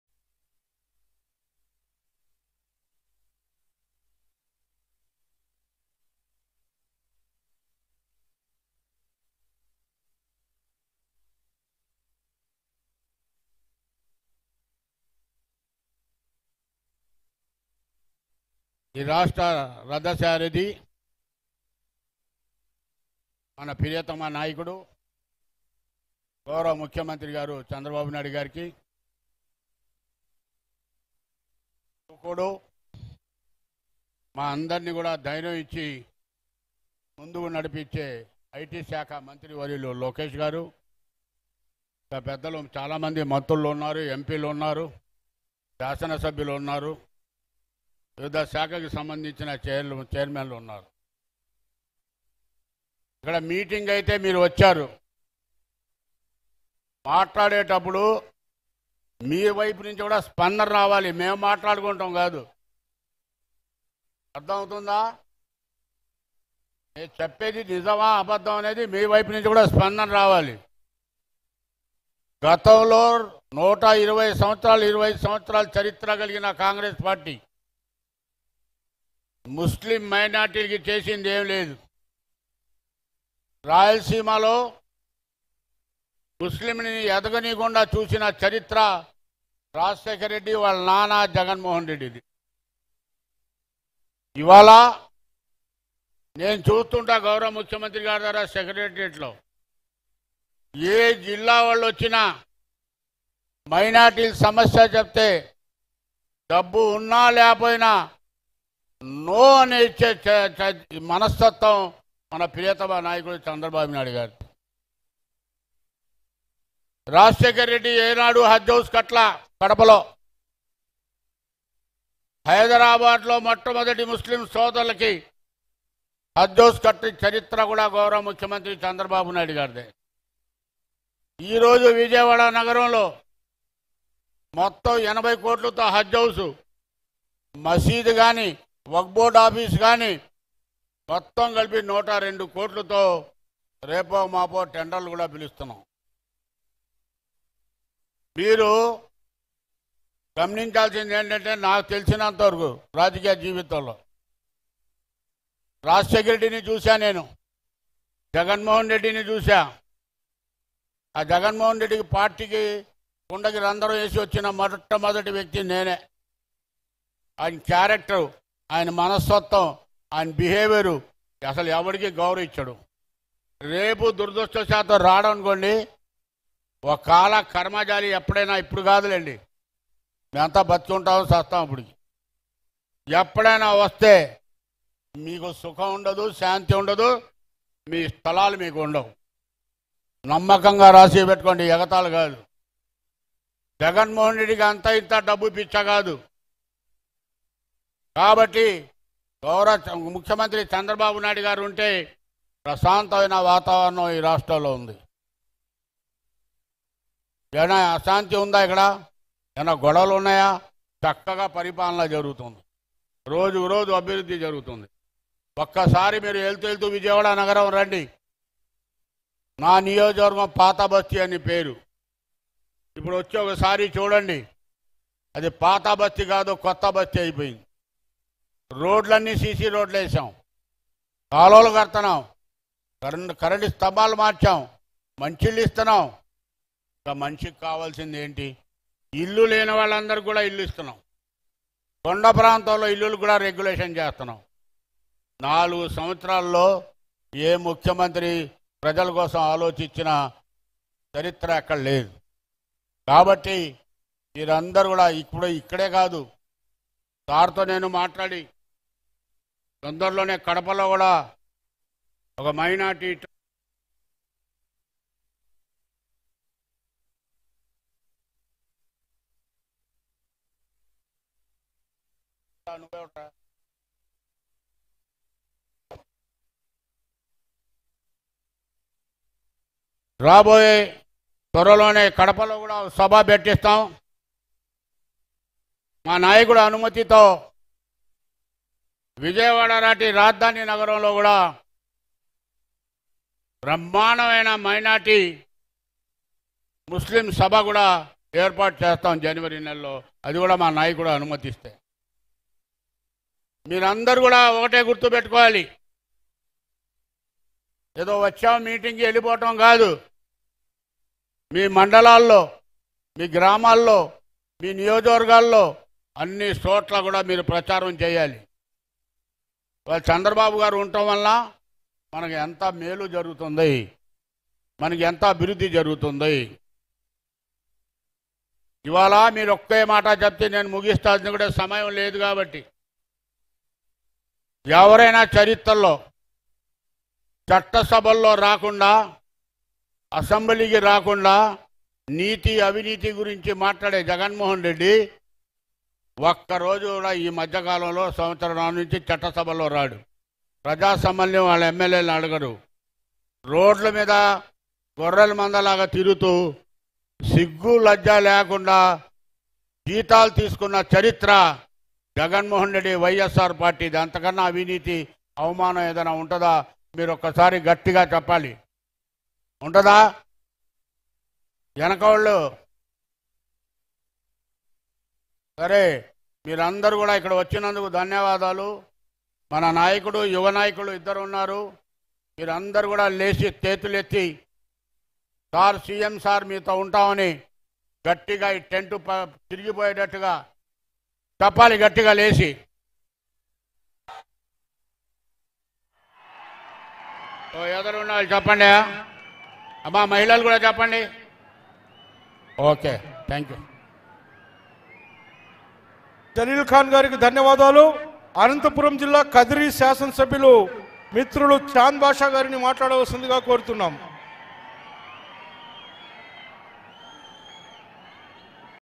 TDP MLA Jaleel Khan Tongue Slip Like Bcom Physics | YS Rajasekhara Reddy S/o JAGAN | YSRCP | YOYO TV
TDP MLA Jaleel Khan excellent Speech in Nara Hamara - TDP Hamara Meeting at Guntur | TDP | YOYO TV
Jaleel Khan Comments on YS Jagan, Taung slip as YSR S/o Jagan like B Com in Physics.
Jaleel Khan is the name of the MLA who reads the physics in B Com. As a party defective MLA. Jalil Khan is always in trouble with his words in the meeting. Jalil Khan, who recently resigned on minority issues in the TDP HAMARA NARA HAMARA, was set up recently in Guntur. Jalil Khan, who started a speech on TDP, said that the Congress and YSRC have been criticized in its style. He has criticized YS Jaganmohan Reddy for criticizing the Congress, who has never had a 125-year-old Congressman in the past. "Rajashekhar Reddy who saw the Muslims in Rayalaseema did not grow up, was their father Jagan Mohan Reddy. As in physics in B Com

సరికొత్త పార్టీ యువ తెలంగాణ అధ్యక్షుడు జిట్లా బాలకృష్ణారెడ్డితో ఎక్స్ క్లూజివ్ ఇంటర్వూ